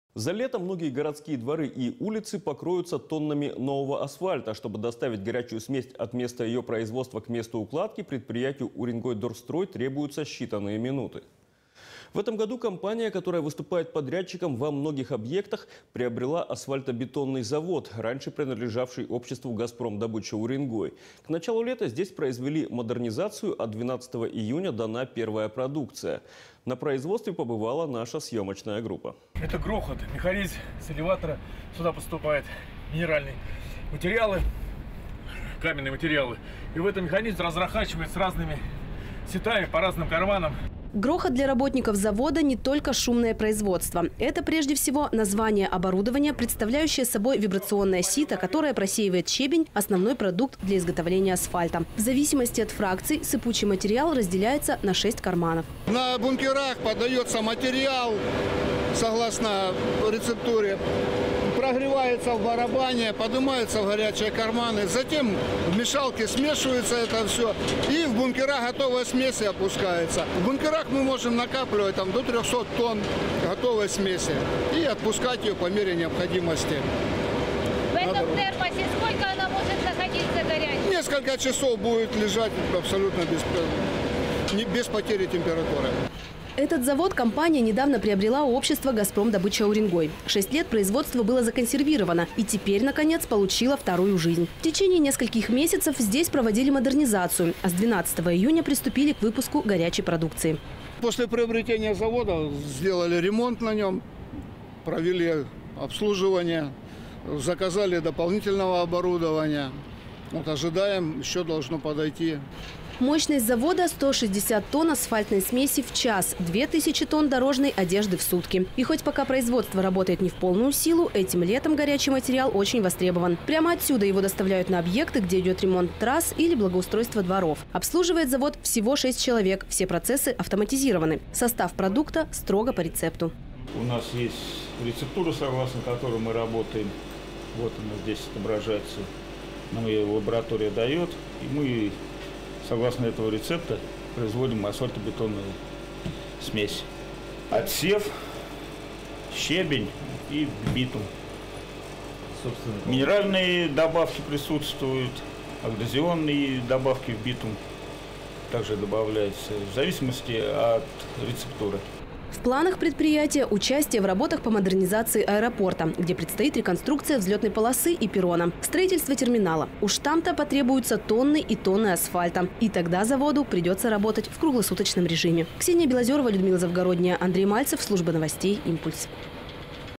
Новый завод компании «Уренгойдорстрой» обеспечивает асфальтом городские улицы и дворы
За лето многие городские дворы и улицы покроются тоннами нового асфальта. Чтобы доставить горячую смесь от места её производства к месту укладки, предприятию «Уренгойдорстрой» требуются считанные минуты. В этом году компания, которая выступает подрядчиком на многих объектах, приобрела асфальтобетонный завод, ранее принадлежавший обществу «Газпром добыча Уренгой». К началу лета здесь произвели модернизацию, а 12 июня дана первая продукция.